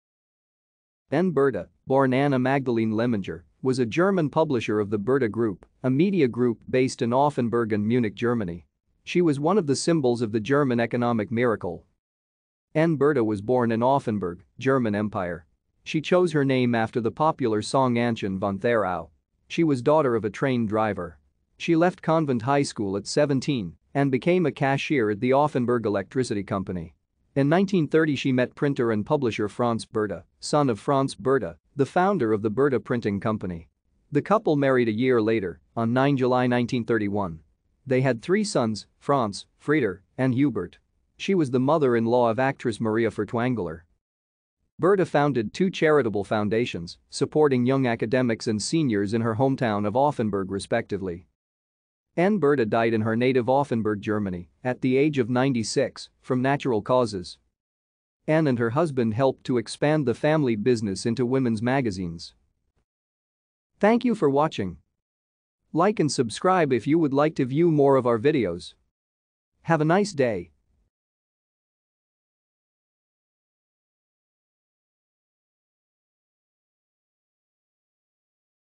Aenne Burda
Aenne Burda, born Anna Magdalene Lemminger, was a German publisher of the Burda Group, a media group based in Offenburg and Munich, Germany. She was one of the symbols of the German economic miracle.